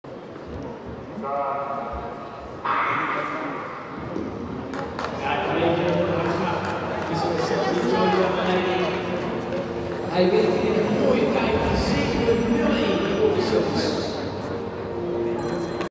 JA 60m 1e serie
Xavi Joy Mo-Ajok blijft met winst en een tijd van 7,01 net boven de 7,00 grens. Jan Roelof Veyer is met 7,16 snel en wordt 2e voor Isiah Emanuelson die 3e wordt. Jesse van Heijningen AV34/Team Sotra noteert 7,35 en plaatst zich hiermee voor de finale